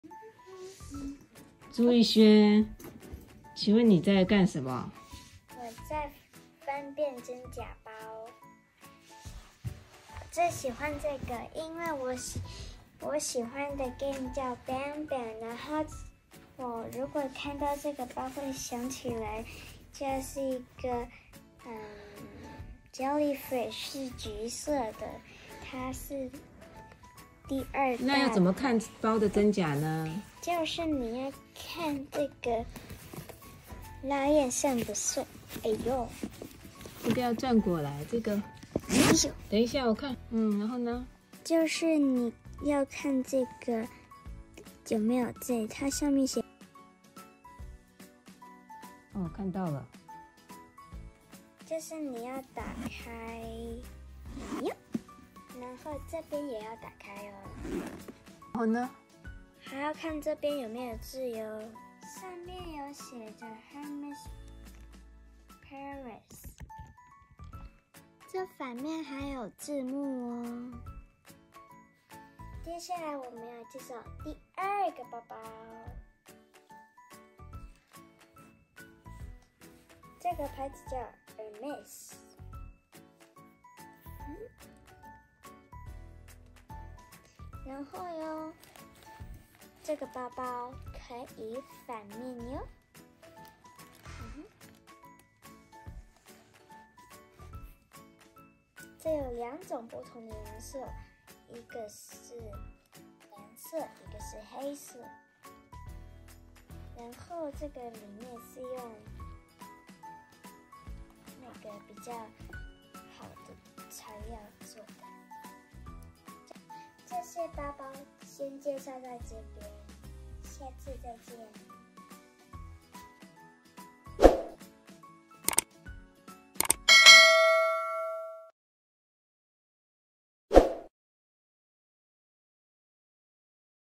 Aaron逸軒教大家如何分辨Hermès愛馬仕真假包包 part2。
Aaron逸軒教大家如何分辨Hermès愛馬仕真假包包。上次跟著媽媽看學會了以後這次被媽媽發現他在玩媽媽的包包，順便跟大家說說怎麼看真假包。搬了一些包出來拿著媽媽的包說我看看這是真的包還是假的，先看看拉鏈順不順暢, 然後去拉包的拉鏈。😄😄 😄

上次Aaron放學回家看到媽媽正在看前一陣子熱烈討論的話題，於是跟著看學得還真快(part 1)！這樣以後幫女朋友買包就知道怎麼看了。Youtube 😄分辨真假